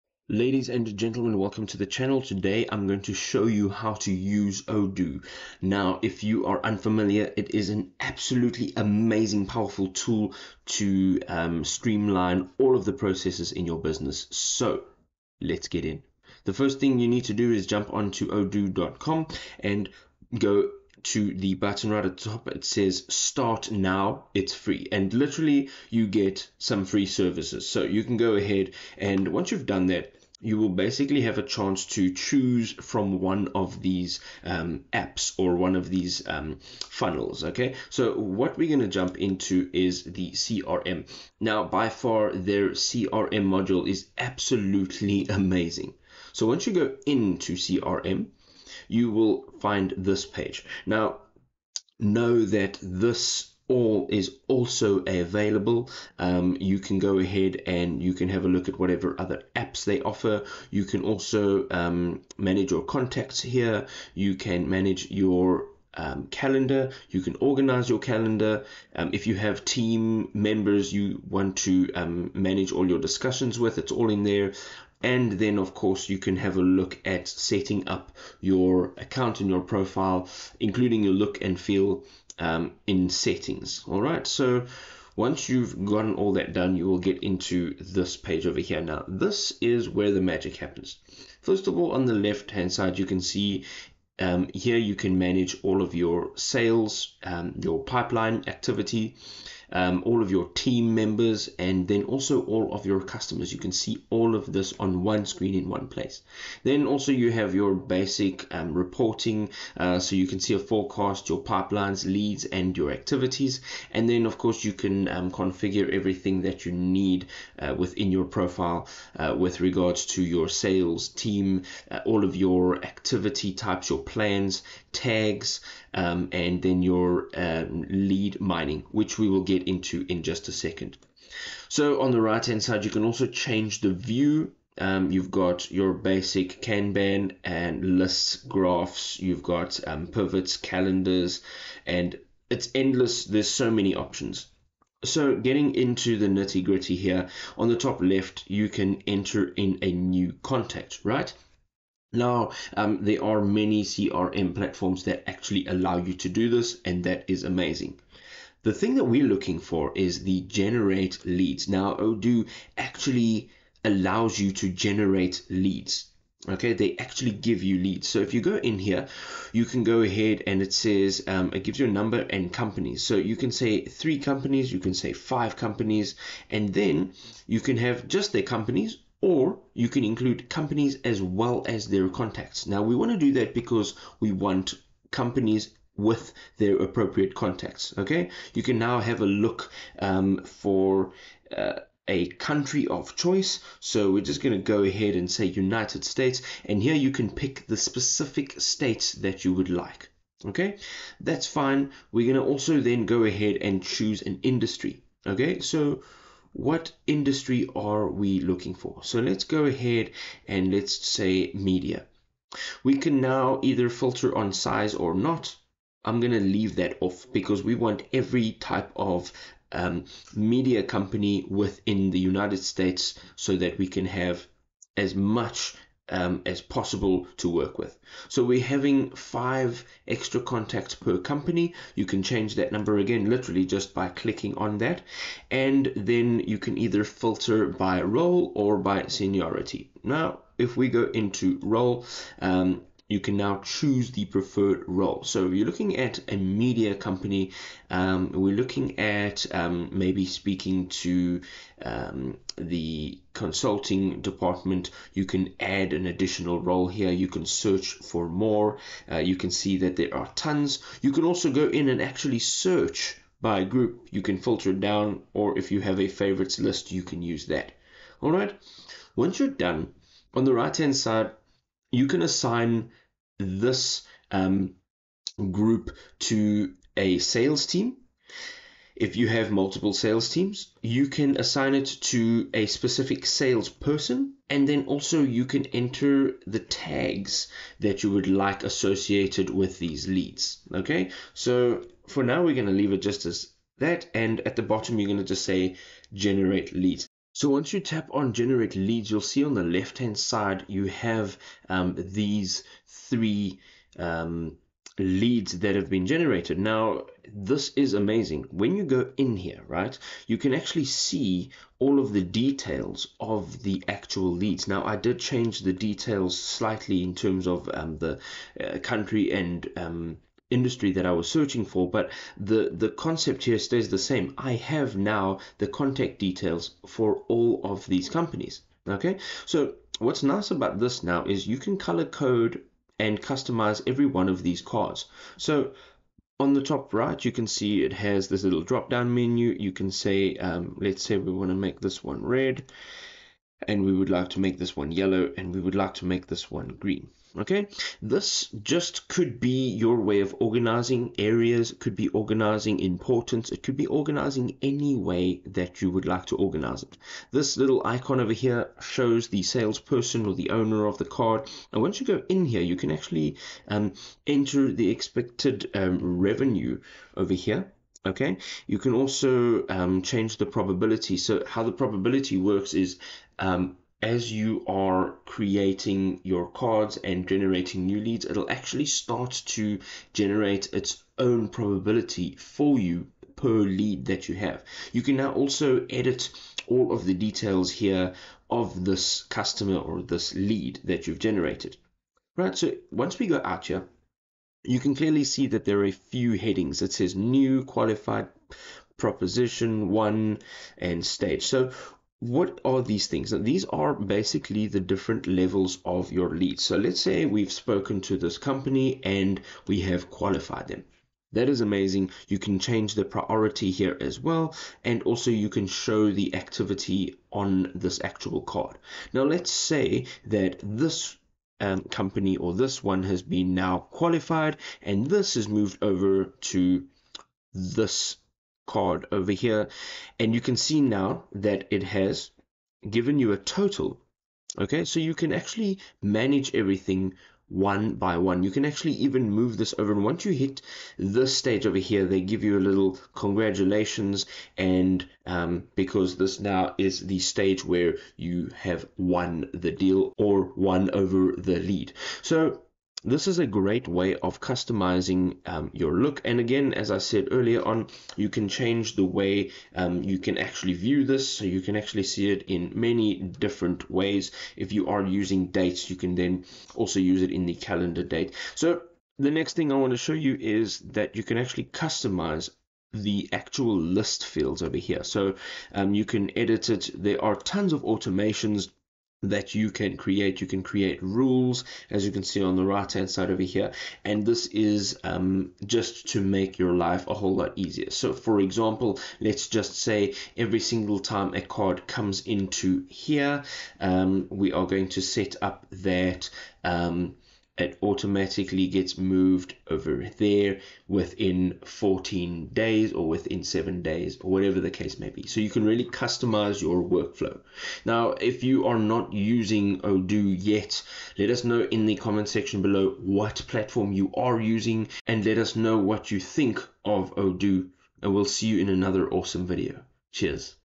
How to Use Odoo | Odoo Tutorial 2025
In this tutorial on 'How to Use Odoo,' we will guide you through the essential steps to effectively utilize Odoo, a comprehensive suite of business applications for managing various aspects of your organization. Odoo offers modules for CRM, sales, inventory, accounting, HR, and more, all integrated into a single platform for seamless operations. In this tutorial, we will cover the basics of navigating the Odoo interface, setting up your account, creating and managing modules, and optimizing your workflow using Odoo's features. Whether you're a small business owner, a project manager, or an entrepreneur looking to streamline your operations, this tutorial will help you harness the power of Odoo to enhance productivity and efficiency in your business processes. Watch now and unlock the full potential of Odoo for your organization's success!

Timestamps

Intro - 00:00 - 00:15
Getting Started with Odoo - 00:16 - 02:40
Generating Leads with Odoo - 02:41 - 05:30
Customizing Your Odoo - 05:31 - 08:29
Conclusion - 08:30 - 08:41